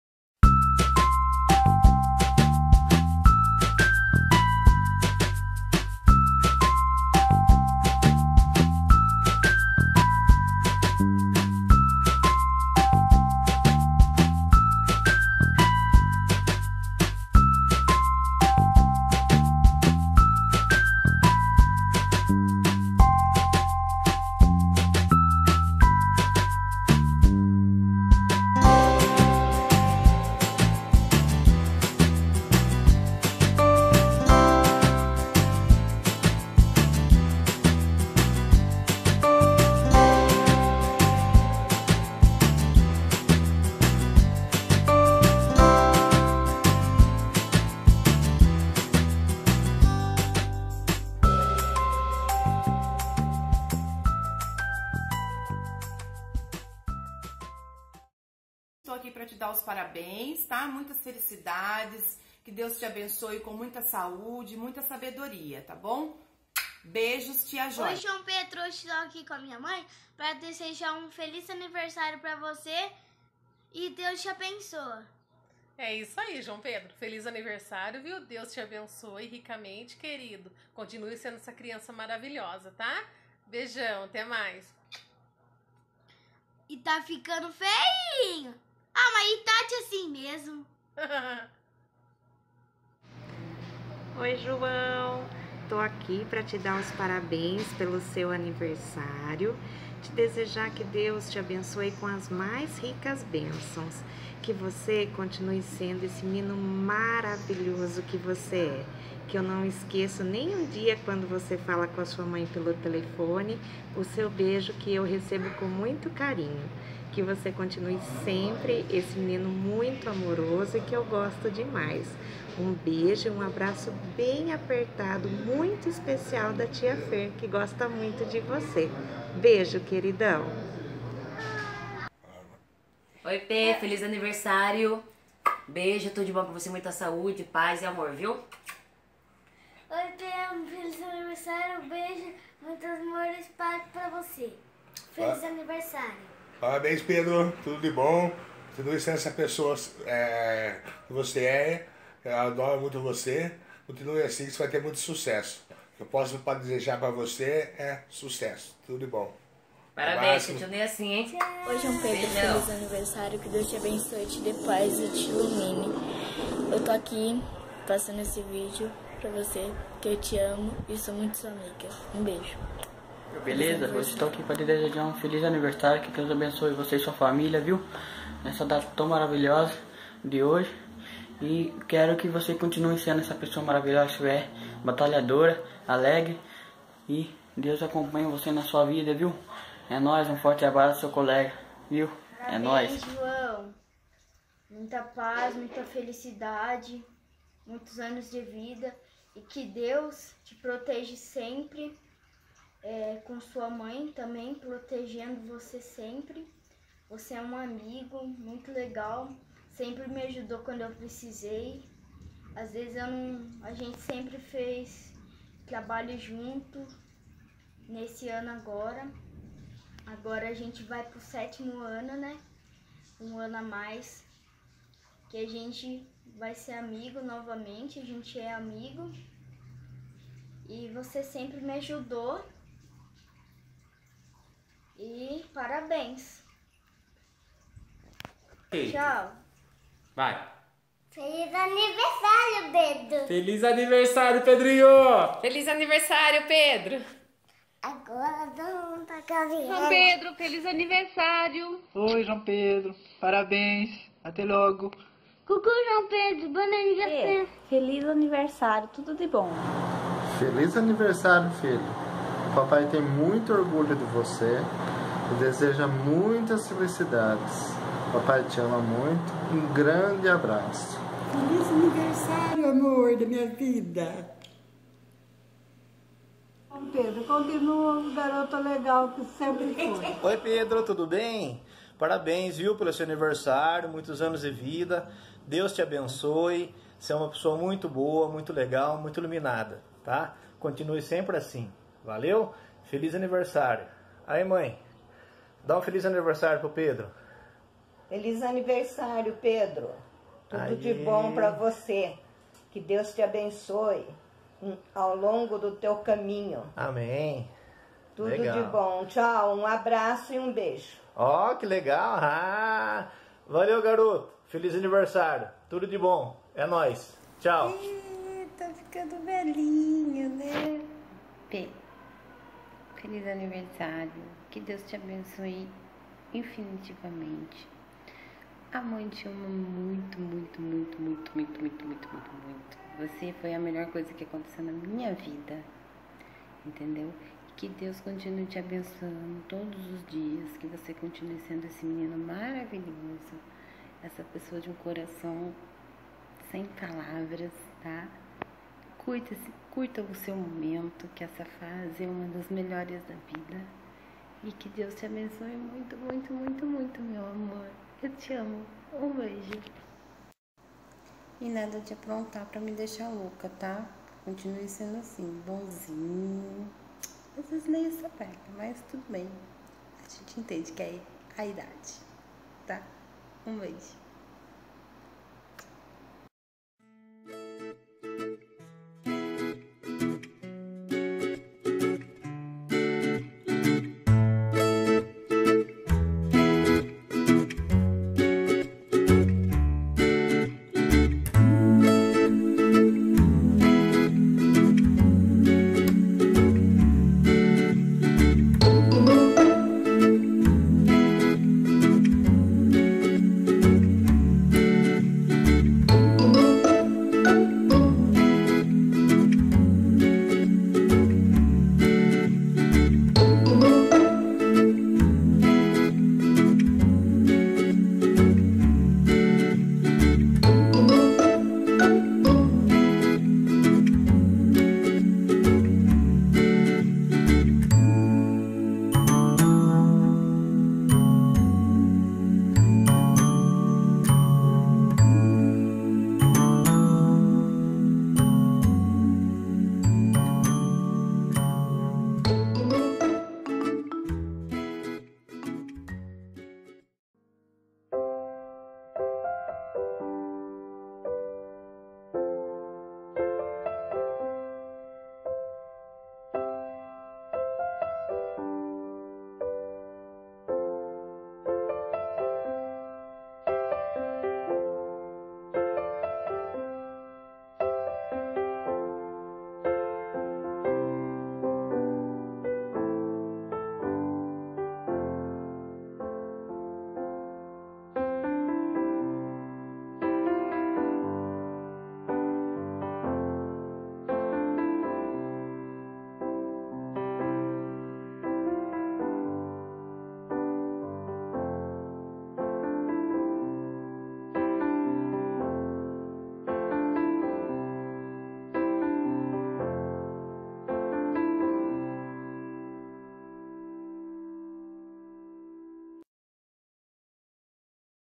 Aniversário do João Pedro
Video especial dedicado para nosso querido João Pedro que completa 13 anos de idade.